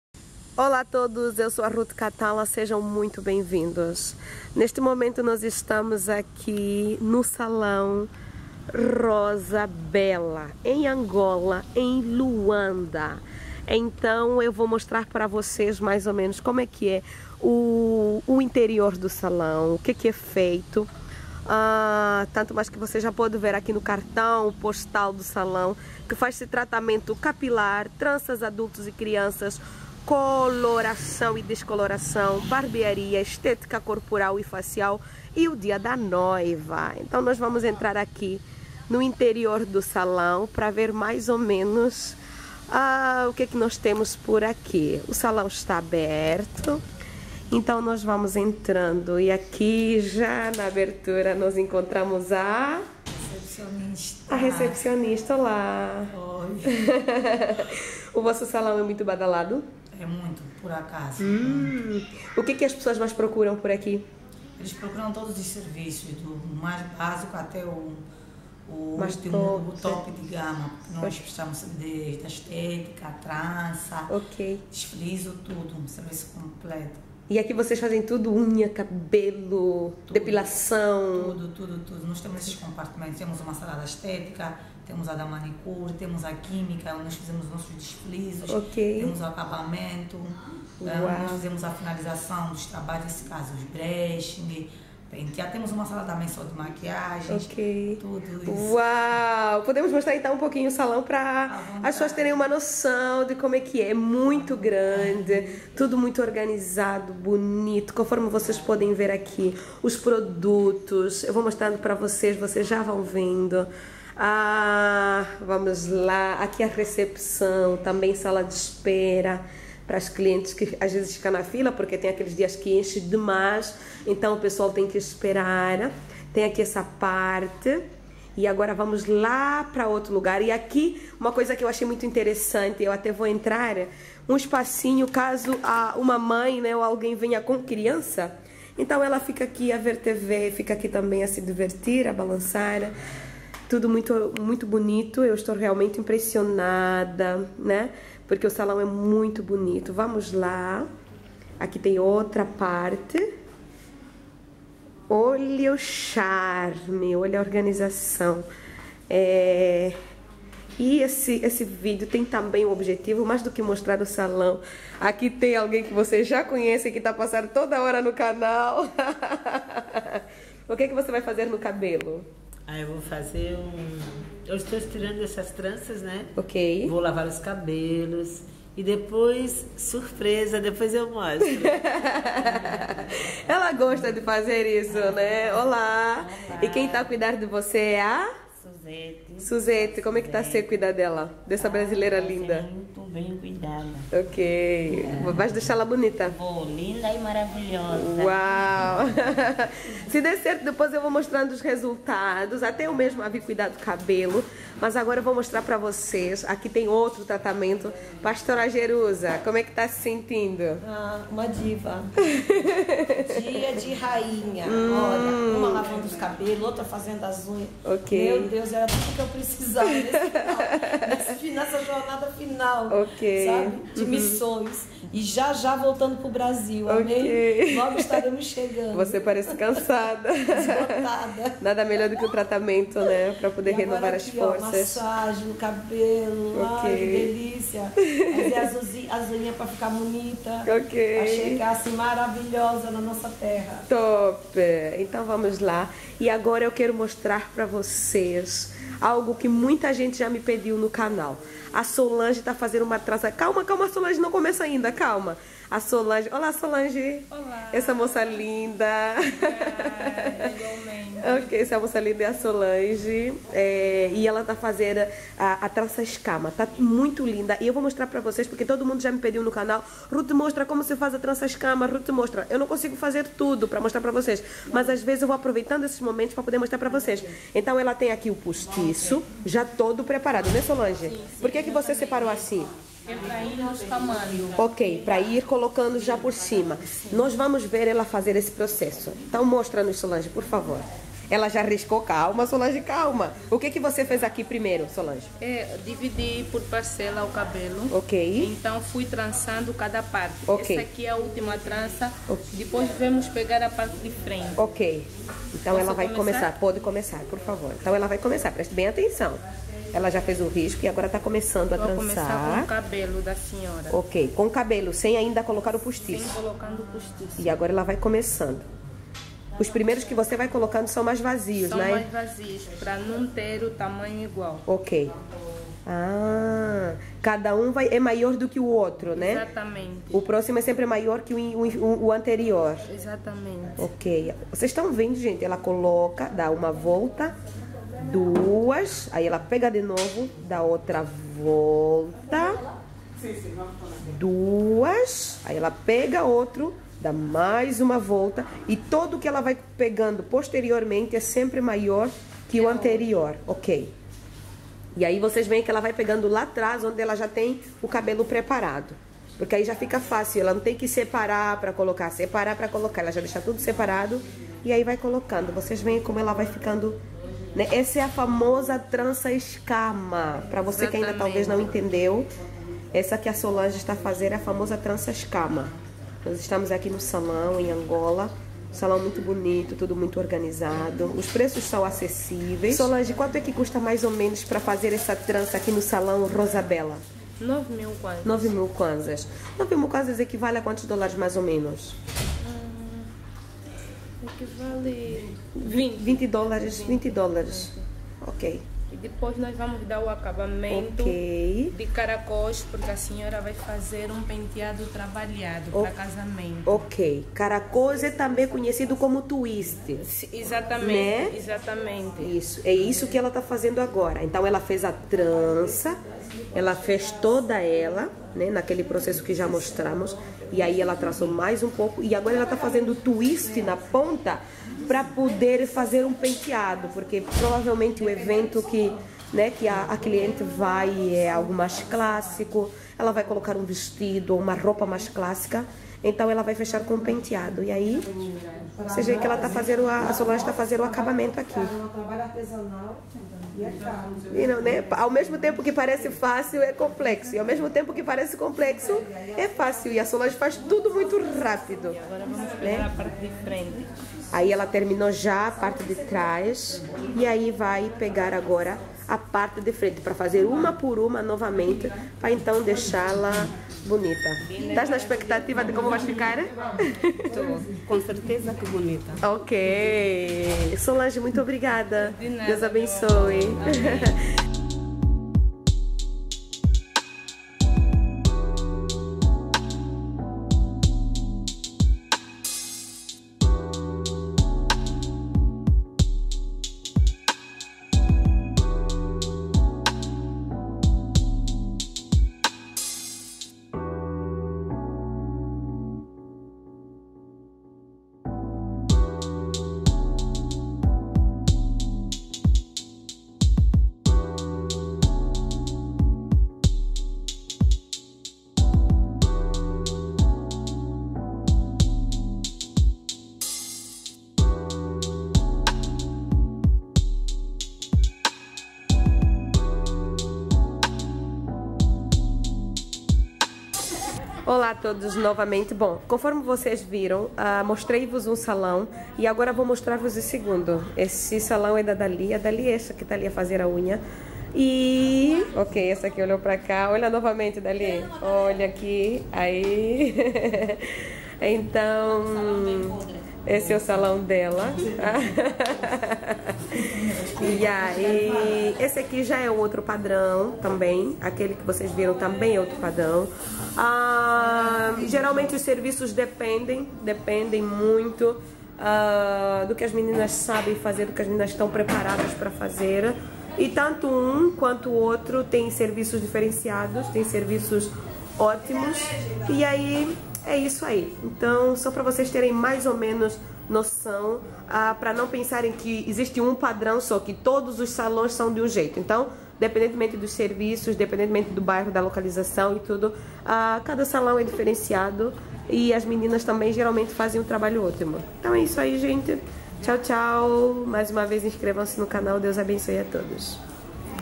Salão de Beleza em Angola; como se faz a Trança Escama; BELEZA AFRICANA
Falar da Cultura africana é falar da beleza angolana nas suas mais diferentes representações.